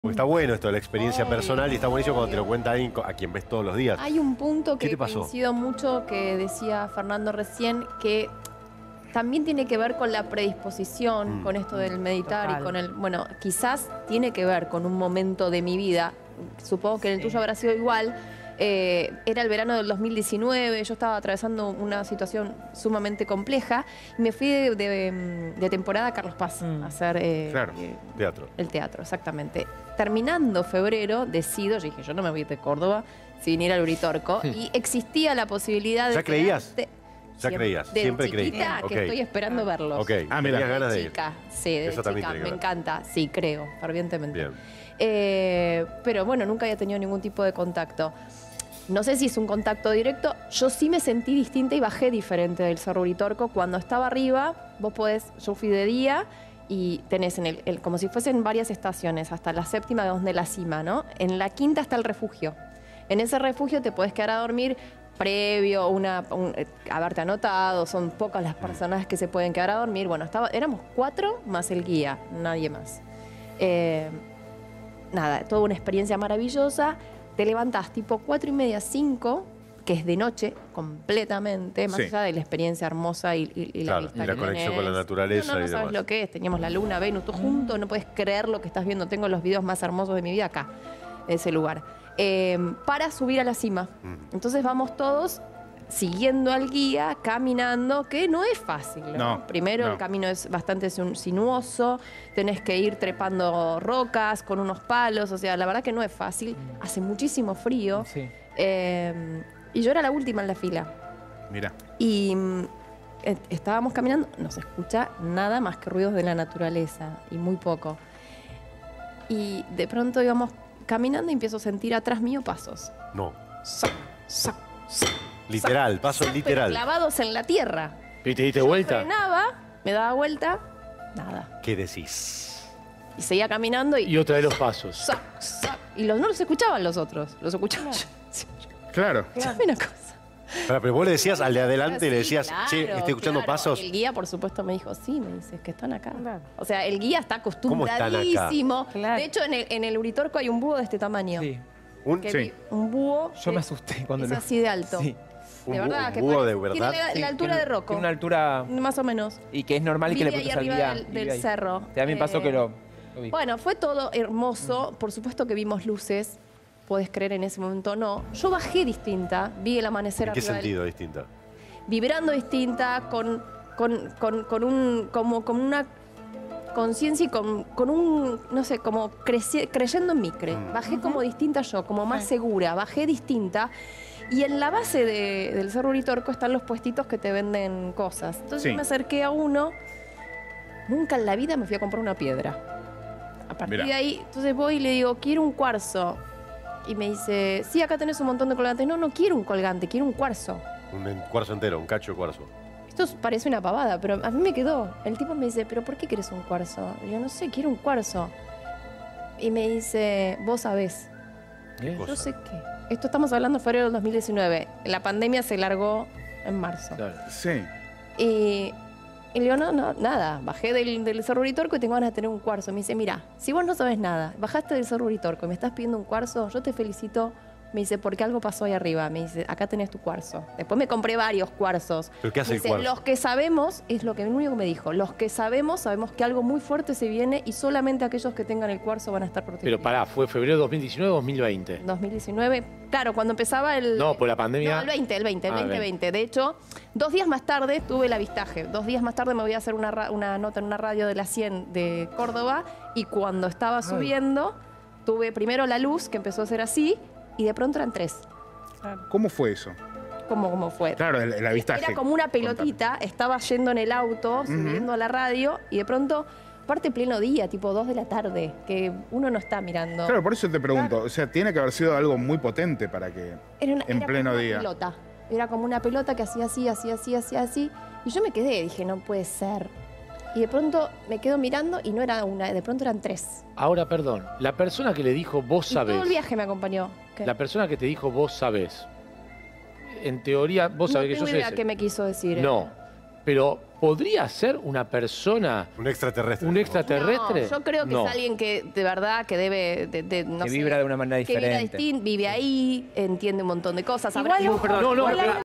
La experiencia paranormal de Julieta Prandi
"Bajé del Cerro Uritorco distinta"
"Tuve un avistaje, era un luz que se movía".

🟩 Encontrá más noticias y todo sobre nuestra programación en nuestra web